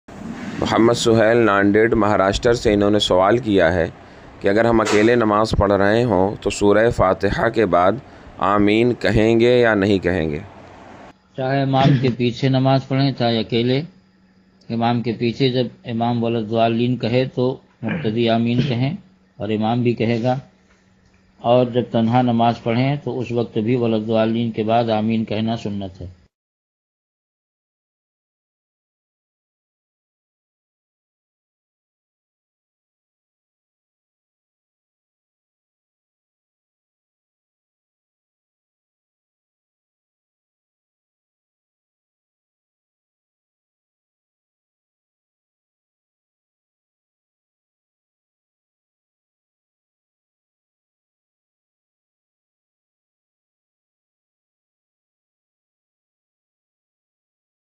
kya Akele Namaz Padhne Wala Surah fatiha Ke Bad Ameen Kahega? By Mufti Khushnood Alam Ahsani Razvi
Kya Akele namaz Padhne Wala Aameen Kahega?
Ans by Khalifa E Huzoor Tajush Shariya Huzoor Shaikh E Millat Hazrat Allama Mufti Muhammad Khushnood Alam Ahsani Razvi Sahab Qiblah Maddazillahul Aali